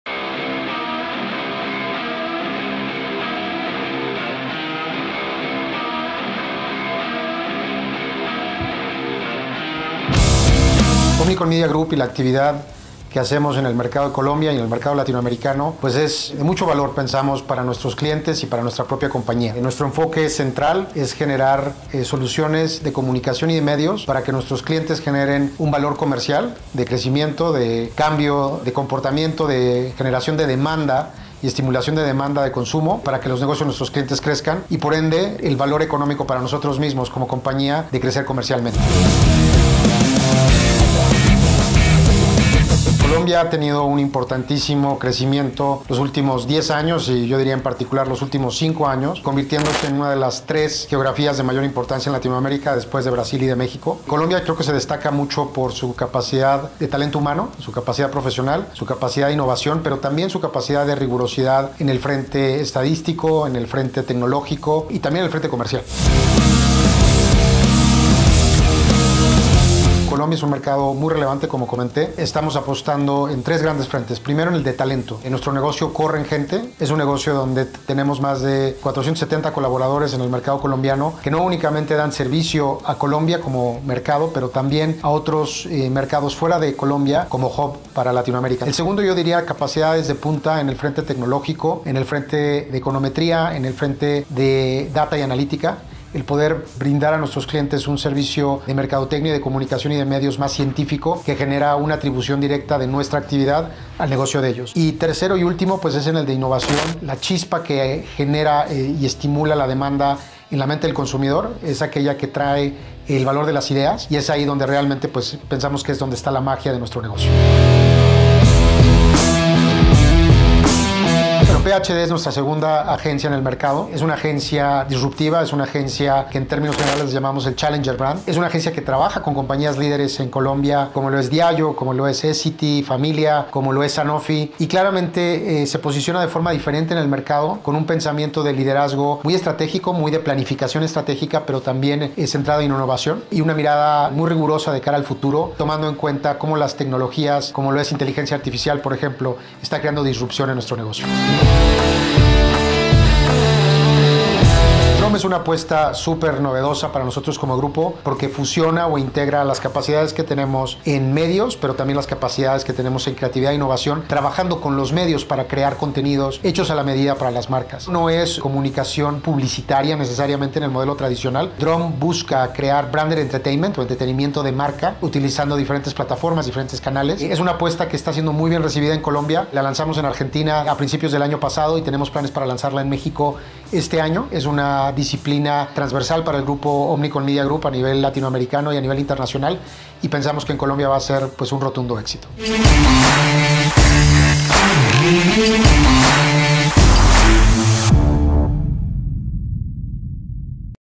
En las oficinas de OMG | Entrevista por Copublicitarias
🆕 ValeLaPenaVer 🇨🇴📹 | Estuvimos en las oficinas de OMG y hablamos con Julián Porras, CEO y nos cuenta cuál es la importancia de una agencia de medios, cómo sigue posicionándose dentro del mercado, y cómo el mercado colombiano desde las unidades de negocio se ha vuelto relevante a nivel Latam. 🤓🙌🏼

También nos cuenta cuál es la apuesta de OMD y PHD en los próximos Q del años a nivel Latam, y por último, con el lanzamiento de DRUM, qué le va a aportar al mercado colombiano. 👏🏼🤩

🎥 Una producción de Xicloide - @xicloide, Agencia Audiovisual
🟢 Presenta Copublicitarias - @copublicitarias